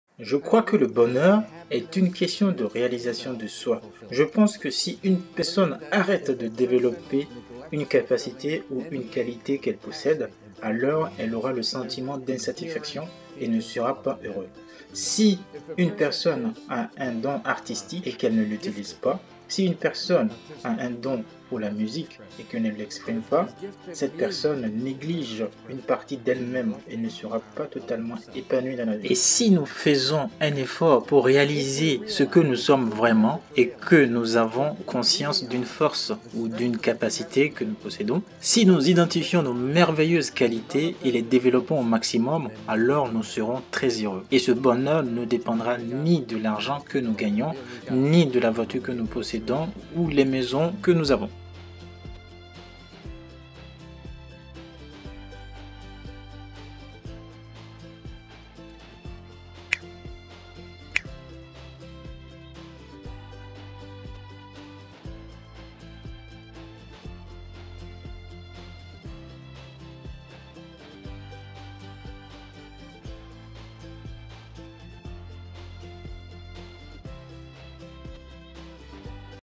Merveilleuse définition du bonheur-Abraham Twerski
Dans cette vidéo Dr Abraham Twerski nous explique la vraie définition du bonheur.

Le vrai bonheur ne dépend d'aucun être,d'aucun objet extérieur.Il dépend de nous même.
Si tu as aimé le contenu, merci de nous soutenir en t'abonnant sur la chaîne.
A bientôt sur la chaîne Agir quantum.